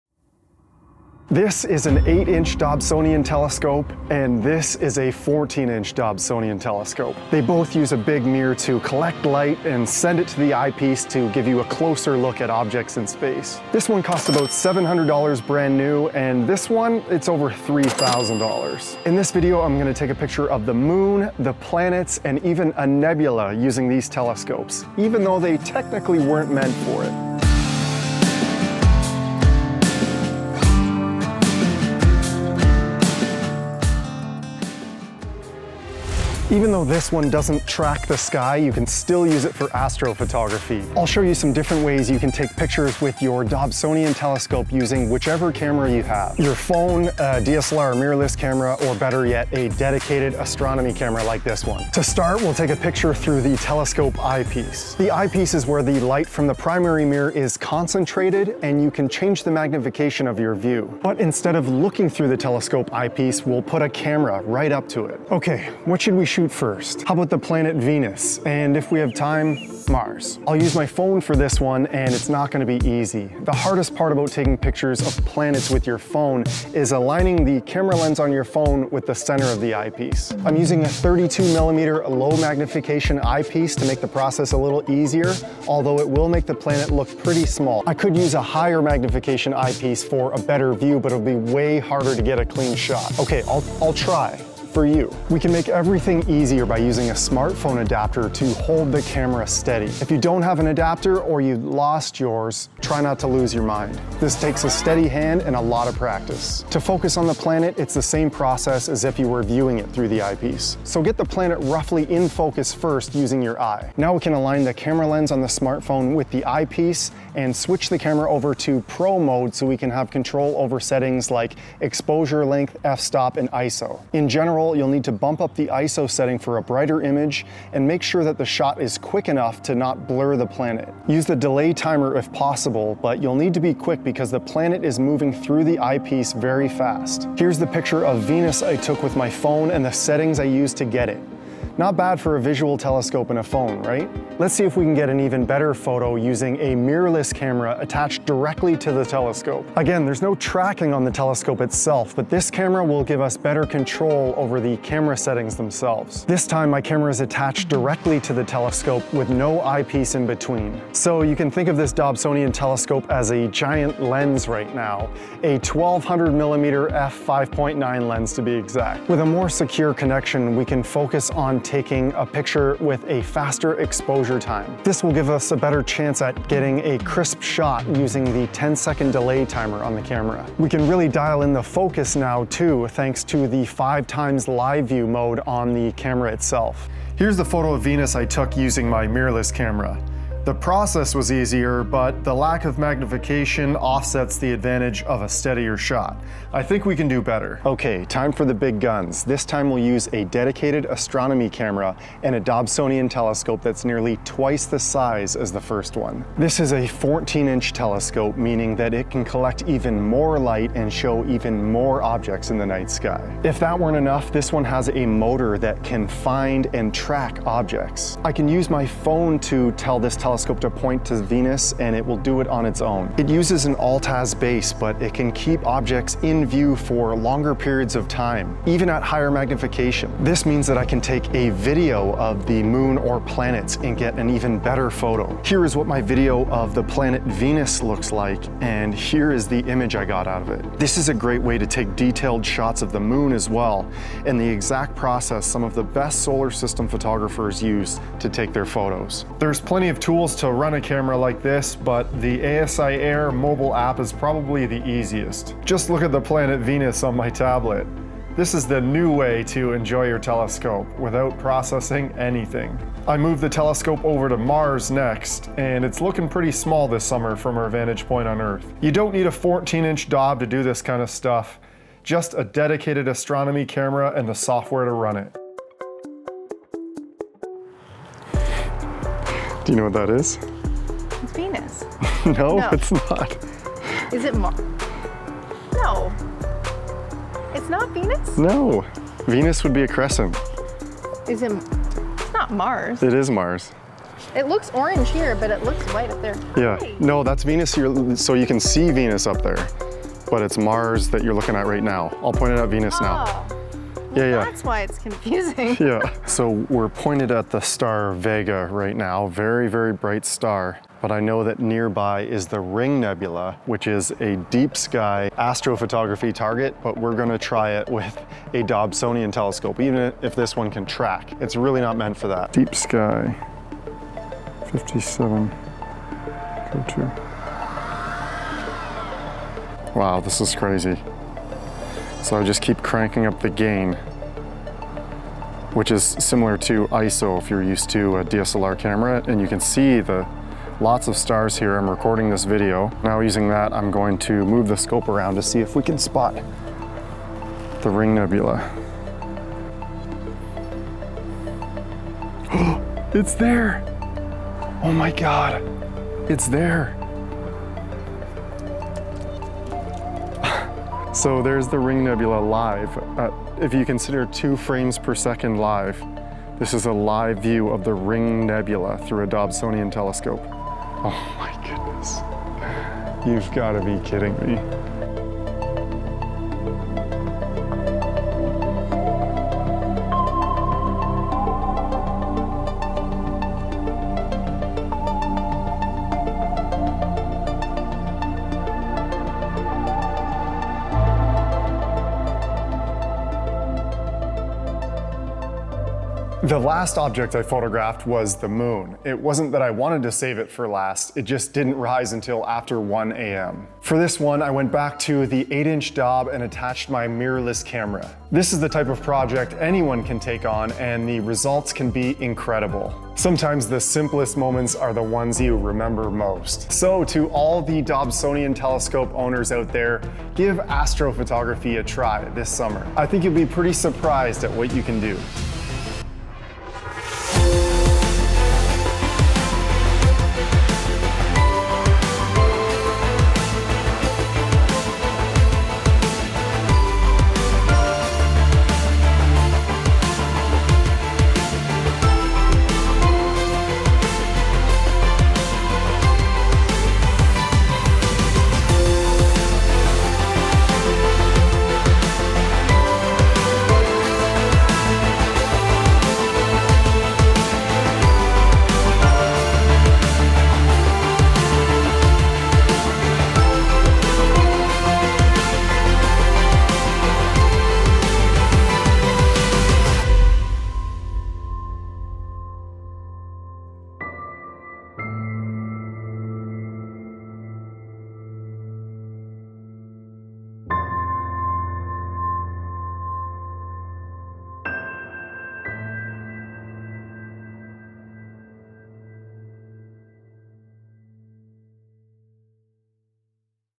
Astrophotography with a DOBSONIAN TELESCOPE!
In this video, I capture Venus, Mars, the Moon, and even the Ring Nebula using a Dob!

While the types of projects you can take on may be limited, there are still plenty of great reasons to use your Dobsonian telescope for astrophotography (even a manual, one!)

YOUTUBE GEAR:

Wide-Angle Zoom Lens
Low-Light Lens